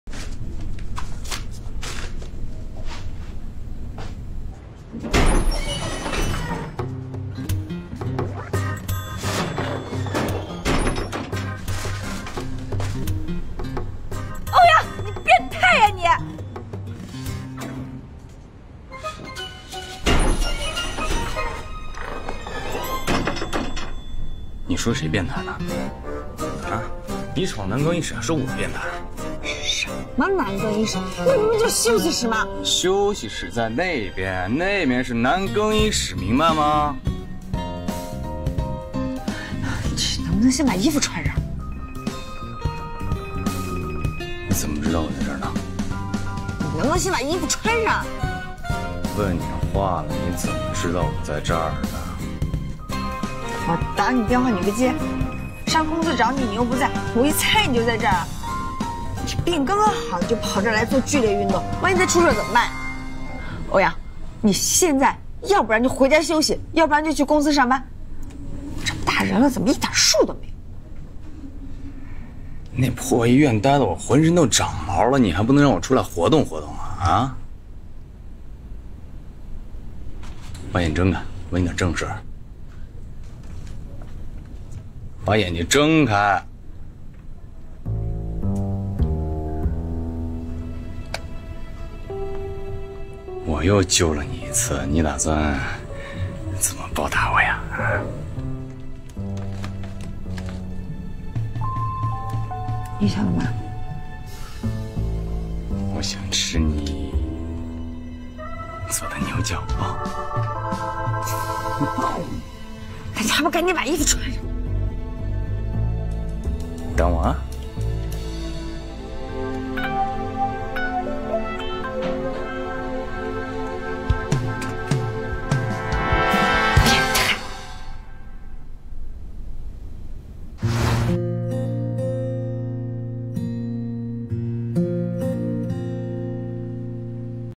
灰姑娘误闯男更衣室，被总裁霸道壁咚，羞得不敢睁眼
第21集

茶茶爱看剧 WYHNZYQ